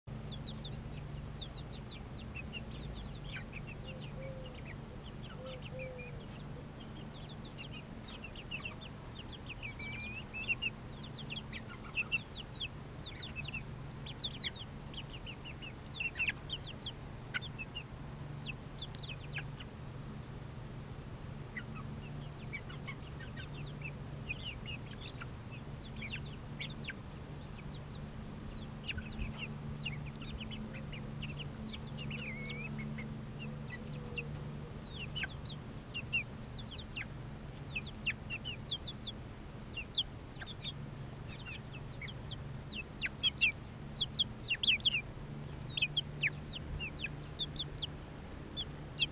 Road's End Treasured Feathers
Banana Peel War- 2.5 months old Polish Bantam chickens